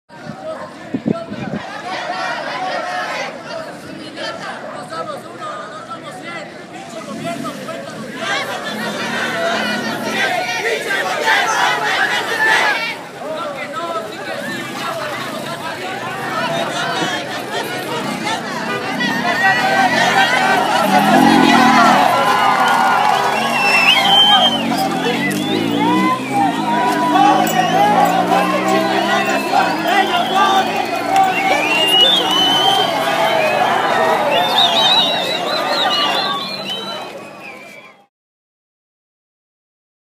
Estudiantes de Prepa 2 SCLC, Chiapas, contra las reformas estructurales. Septiembre 27, 2013
Estudiantes de la preparatoria 2 de San Cristóbal de Las Casas, se manifiestan contra las reformas estructurales. Marcha que concluyó en el zocalo de la ciudad en el marco de la lucha magisterial contra la reforma educativa.